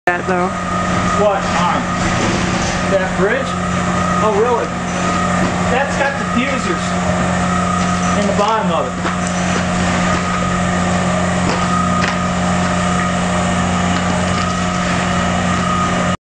Schreiber CAS.AVI
Schreiber Countercurrent Aeration System -- Activated Sludge basin with unique aeration and mixing system.